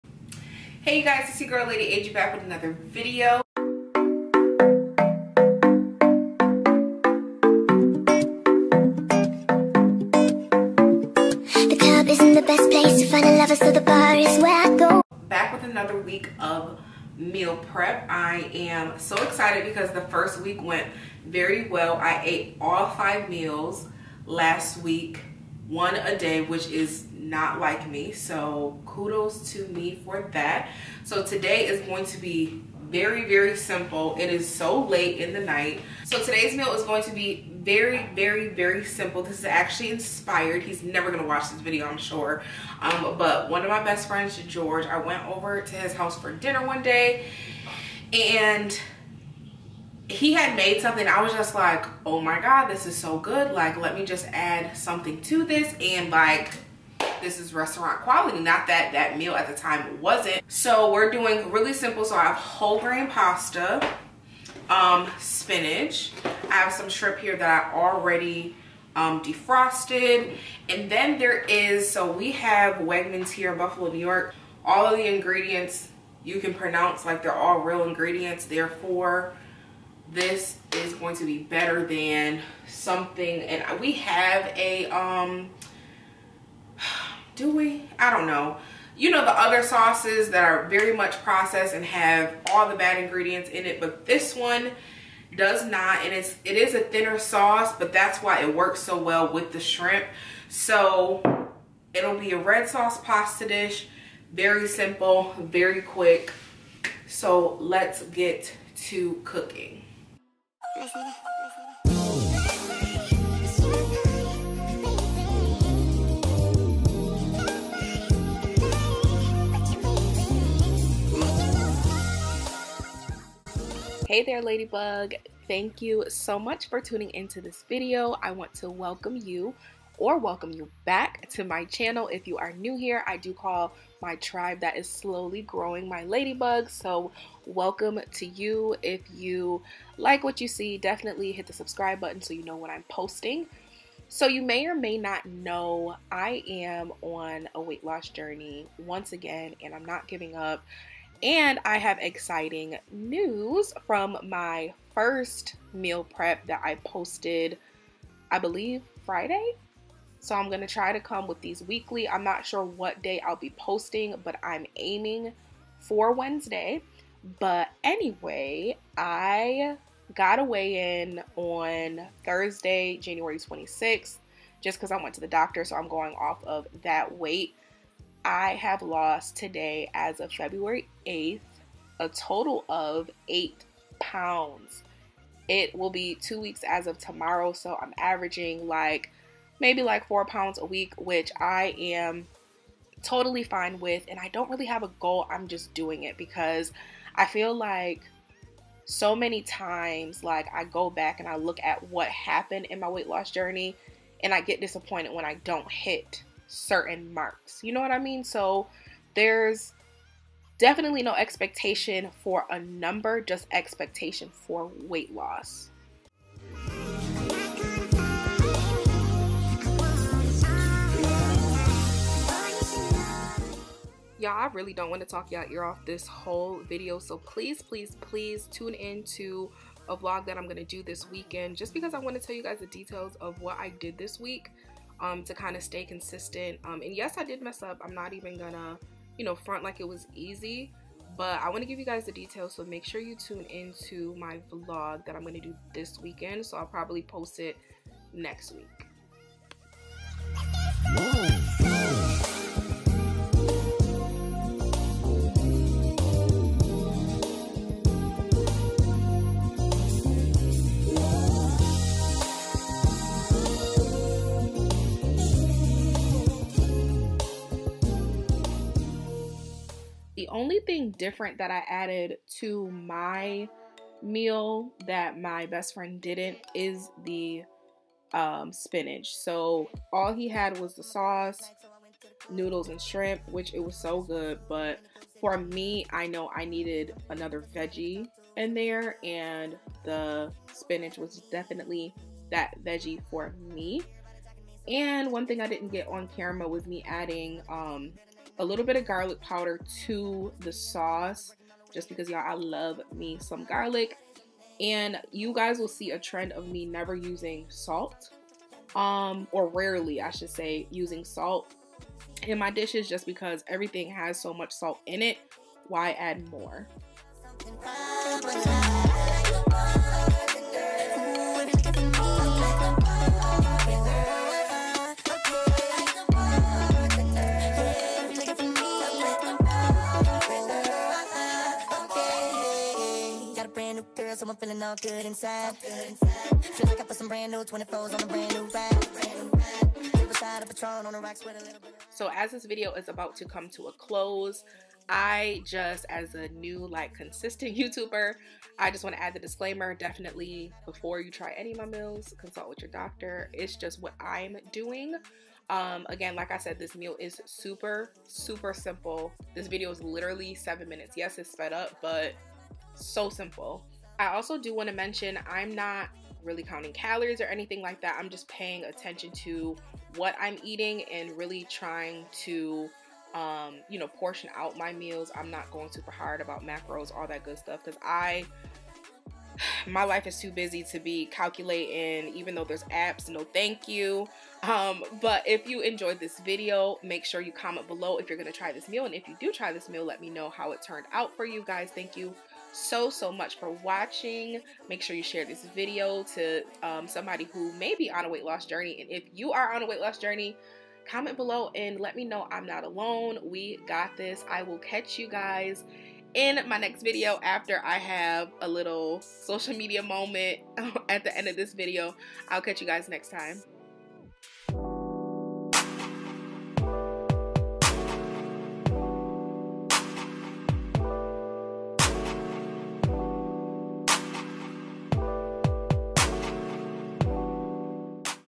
MEAL PREP WITH ME | WEIGHT LOSS JOURNEY | STAYING CONSISTENT
Hey Ladybug!! Welcome back to another video!! You'll love it here! PS... Make sure you say hey in the comments so I know you're watching!

Welcome to week 2 of meal prepping!!! Today's meal was SUPER easy!! Y'all the first week was a challenge... but I made it!  Like I mentioned in the video, I will be doing a vlog this weekend to explain everything that I did during the week. Good luck with week 2! WE GOT THIS!